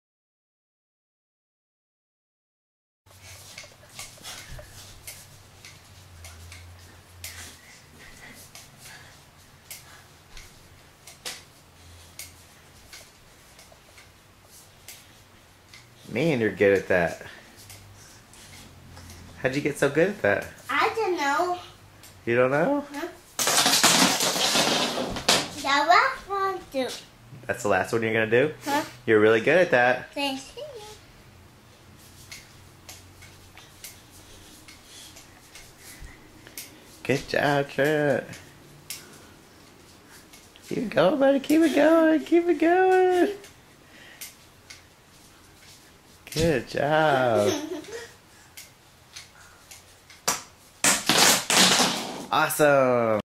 Fruitloops & Hula Hoops-YouTube.mov
Trents new favorite thing to do after breakfast ... as you can see  ... he is wearing his ... but this was to cool not to share ... take care ... Mike ;0 }